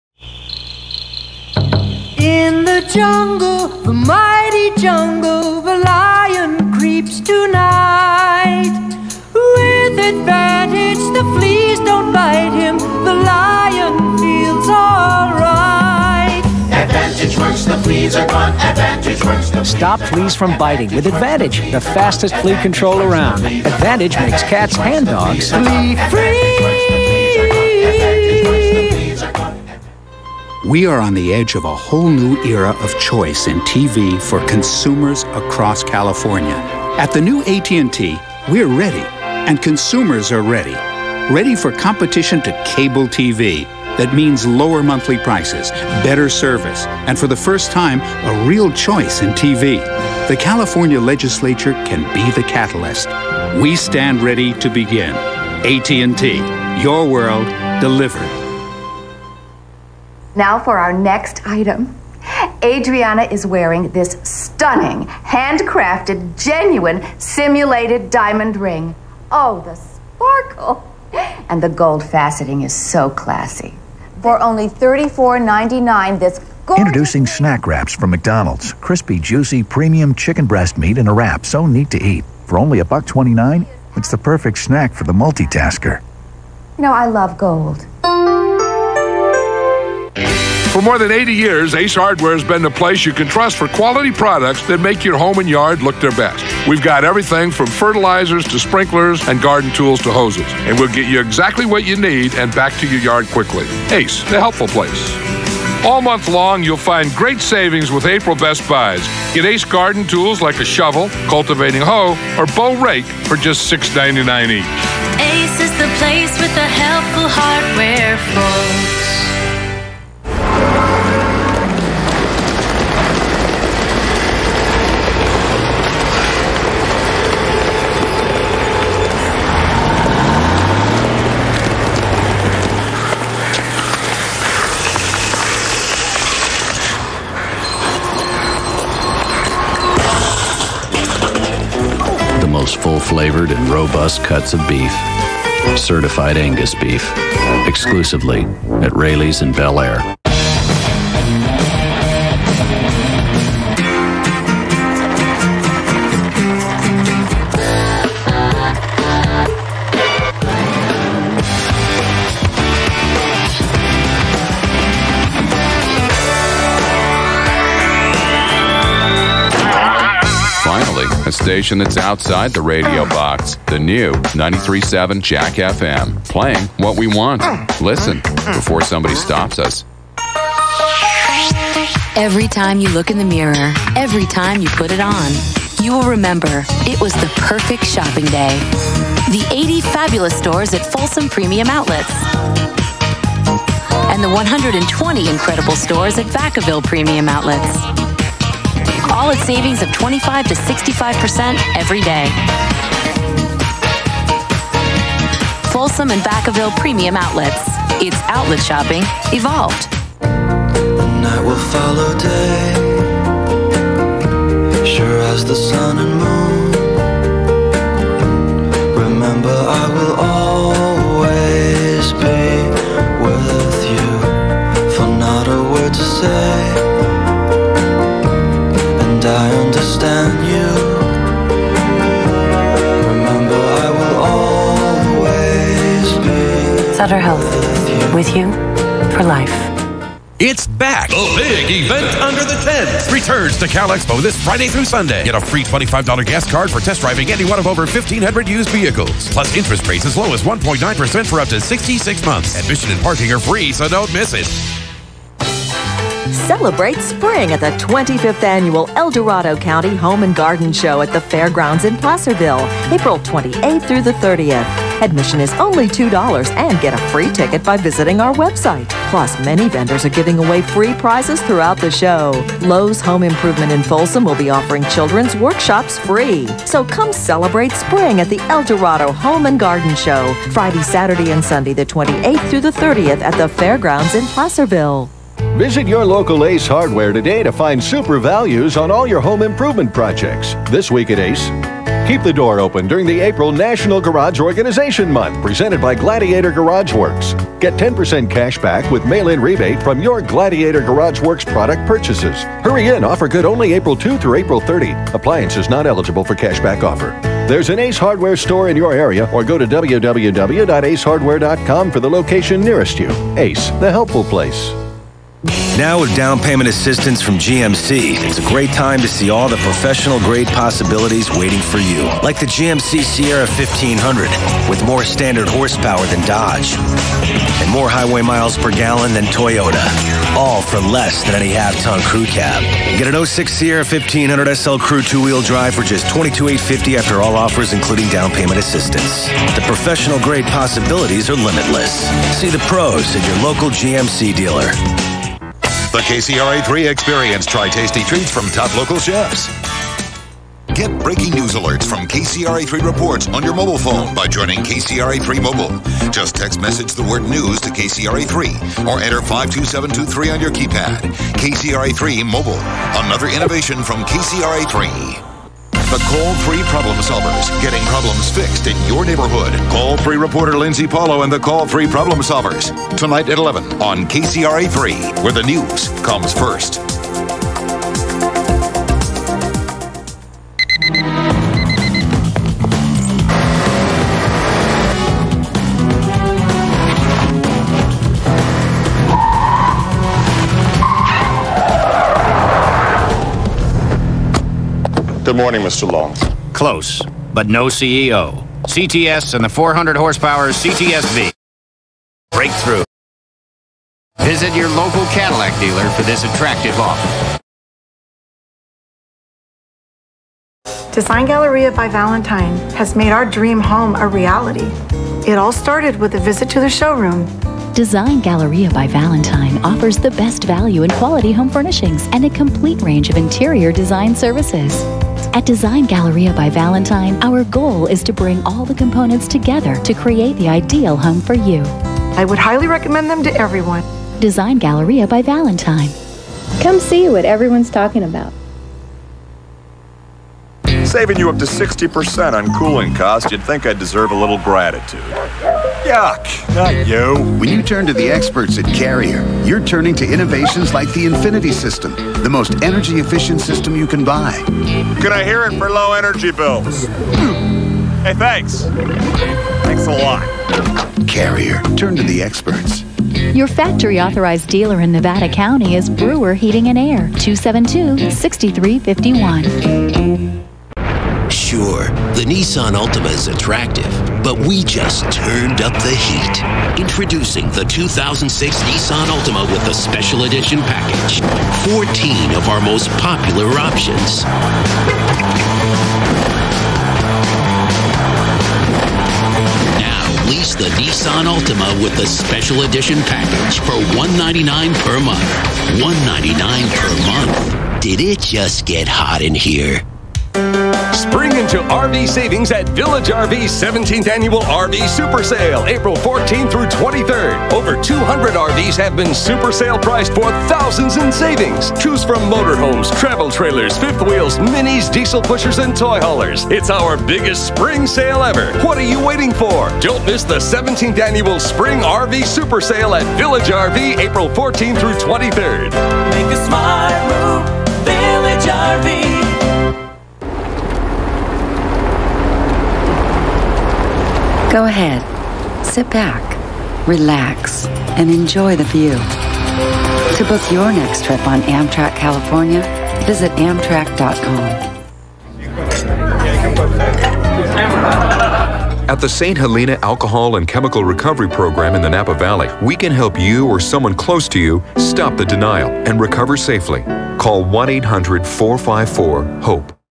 KCRA 3 Sacramento Commercials 4/20/2006 pt. 3
2006 TV Commercials, some local to Sacramento CA